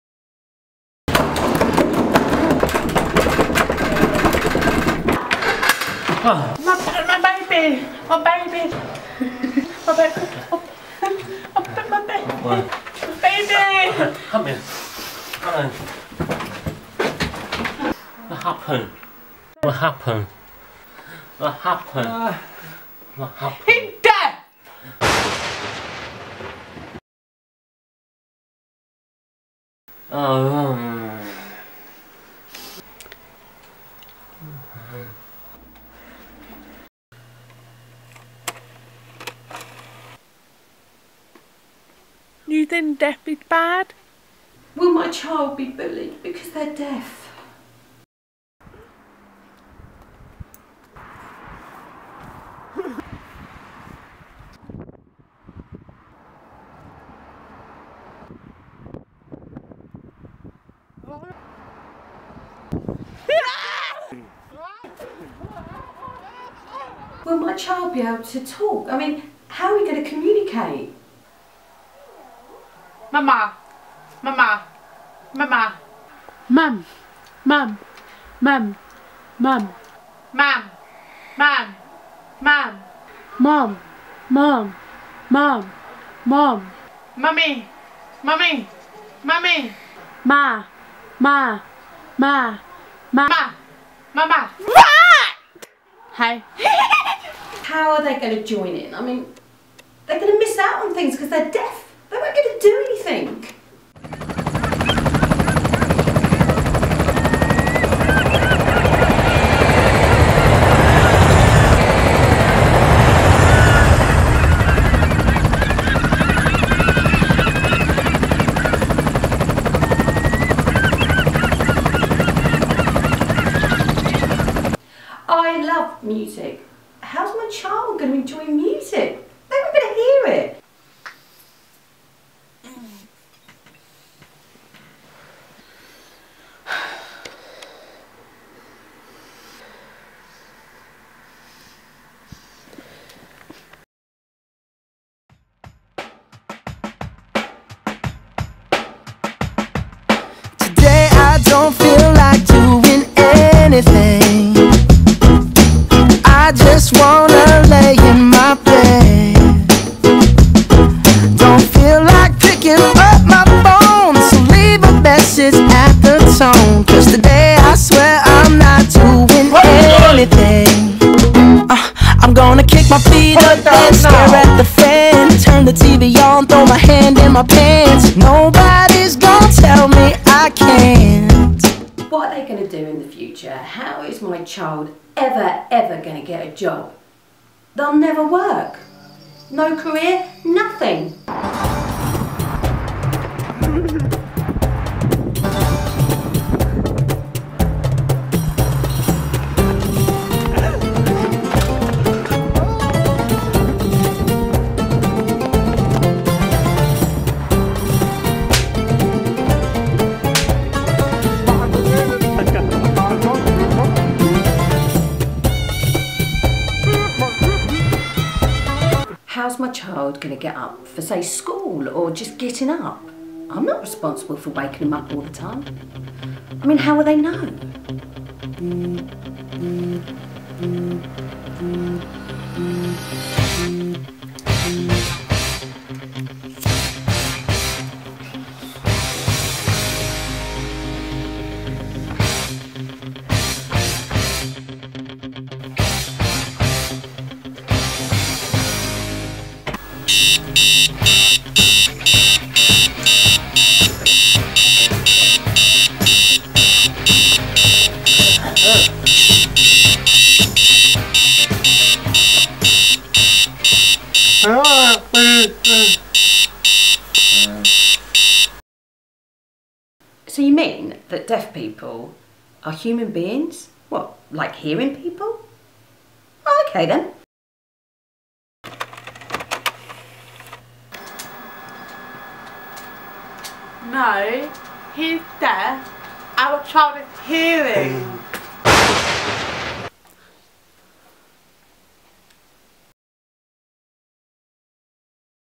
Deaf Ability
My child is deaf (don't panic!). A light-hearted look into any concerns you may have.